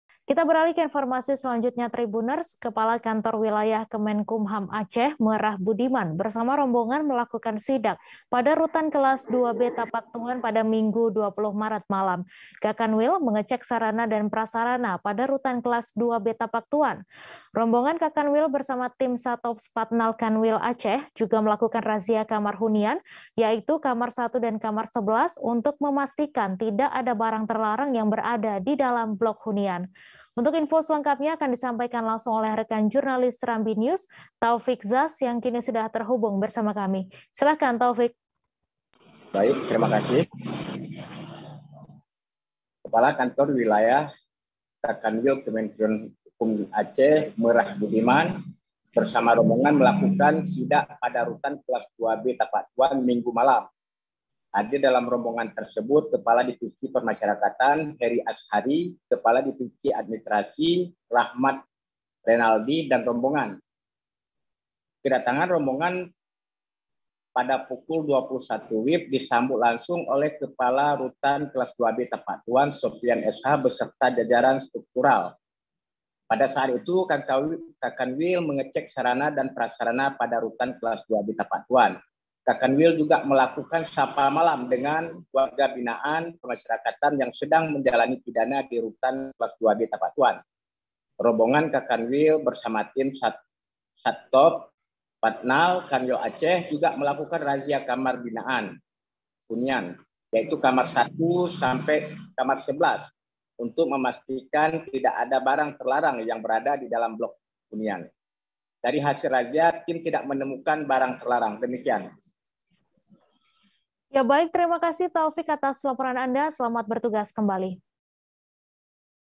Kemenkumham Aceh Sidak Rutan Kelas IIB Tapaktuan
SERAMBINEWS.COM, TAPAKTUAN - Kepala kantor wilayah (Kakanwil) Kemenkumham Aceh, Meurah Budiman SH MH, bersama rombongan melakukan sidak pada Rutan Kelas IIB Tapaktuan Minggu (20/03/2022) malam. Hadir dalam rombongan tersebut Kepala Divisi Pemasyarakatan, Heri Azhari, Kepala Divisi Administrasi, Rakhmat Renaldi dan rombongan.

Kedatangan rombongan pada pukul 21.00 WIB disambut langsung oleh Kepala Rutan Kelas IIB Tapaktuan, Sofyan SH beserta pejabat struktural (Kepala Pengamanan Rutan dan Kasubsi Pelayanan Tahanan).

Pada saat itu Kakanwil mengecek sarana dan prasarana pada Rutan Kelas IIB Tapaktuan, Kakanwil juga melakukan sapa malam dengan WBP yang sedang menjalani pidana di Rutan Tapaktuan,

Rombongan Kakanwil bersama tim Satops Patnal Kanwil Aceh juga melakukan razia kamar hunian, yaitu kamar 1 dan kamar 11, untuk memastikan tidak ada barang terlarang yang berada didalam blok hunian.

Dari hasil razia, tim tidak menemukan barang terlarang.(*)